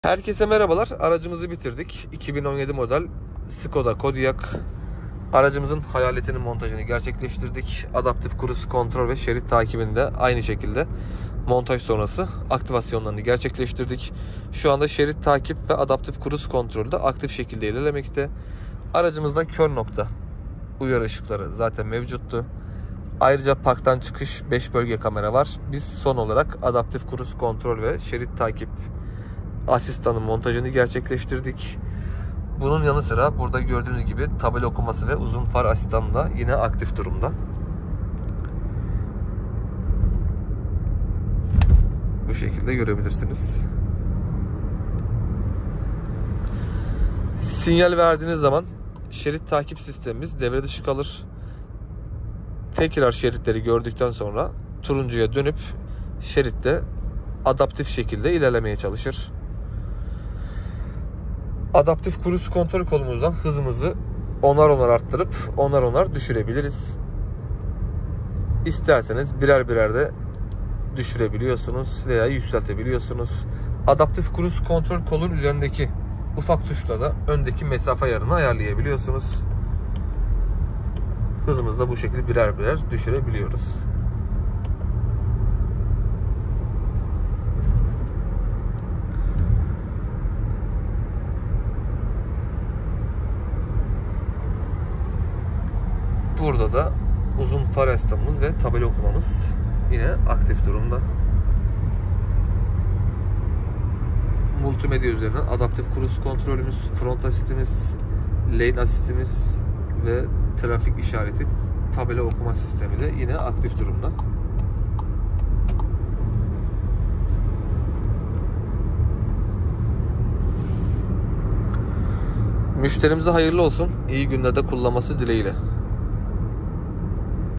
2017 MODEL SKODA KODİAQ ARACIMIZA ADAPTİF HIZ SABİTLEME ŞERİT TAKİP ASİS. VE HAYALET KADRAN MONTAJI
🧨🧨2017 MODEL SKODA KODIAQ RETROFIT APP.💣💣

👉Montajı Yapılan Donanımlar ACC + LANE ASSİST

1️⃣ ACC
✅Adaptif Cruise Control
✅Ön Bölge Çarpışma Önleyici

2️⃣LANE ASSİST
✅Şerit Takip Asistanı
✅Uzun Far Asistanı
✅Tabela Okuma
✅Ani Frenleme

👉ACC VE LANE ASSİST BERABER MONTE EDİLDİĞİNDE EKSTRA İKİ ÖZELLİK GELMEKTEDİR...

✅Trafik Sıkışıklığı Asistanı
✅Acil Durum Asistanı

3️⃣DİJİTAL HAYALET KADRAN

MONTAJ VE KODLAMASI YAPILMIŞTIR…